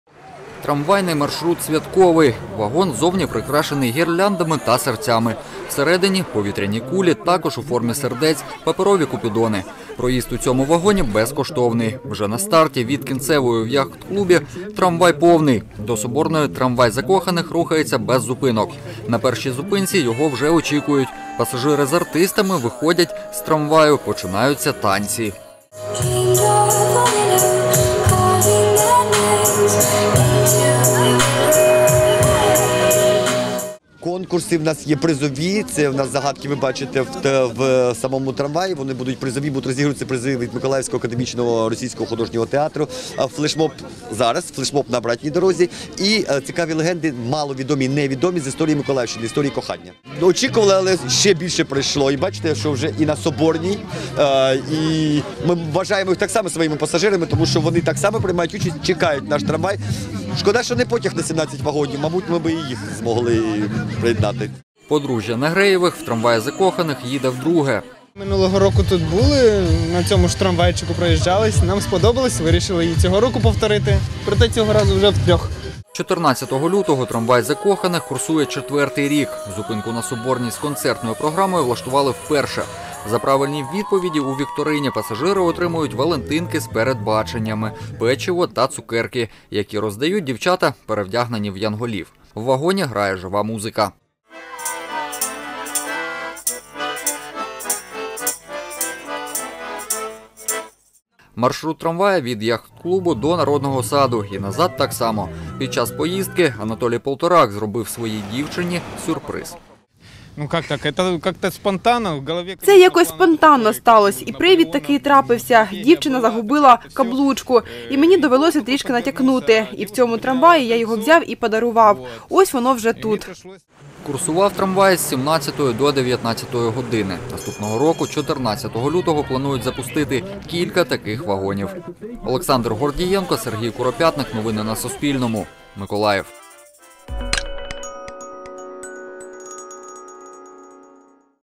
Миколаєвом курсував “Трамвай закоханих”
У Миколаєві в День усіх закоханих курсував тематичний трамвай.  Для пасажирів підготували живу музику, вікторини та шоу-програму.

Автор - Олександр Гордієнко, оператор - Сергій Куропятник.